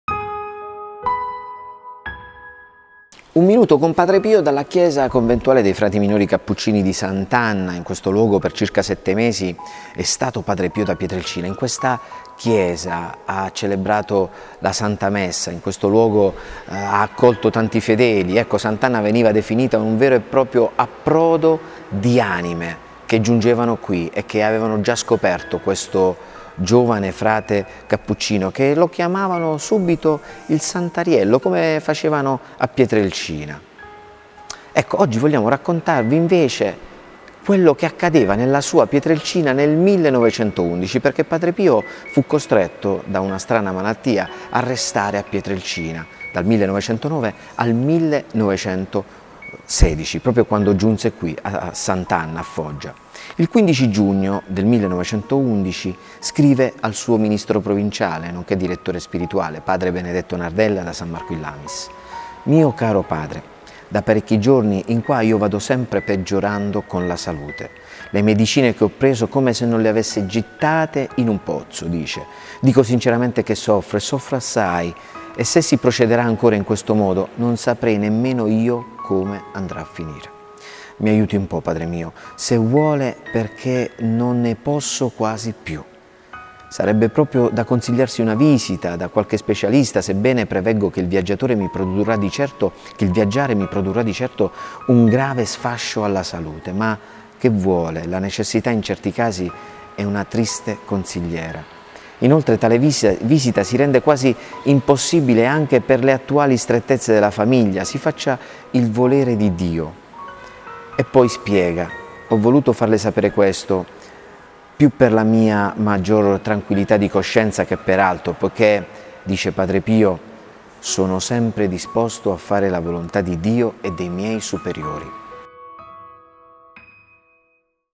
Padre Pio: "mi aiuti padre mio, non ne posso più"
Nel 1911 Padre Pio scrive al suo provinciale sulla sua salute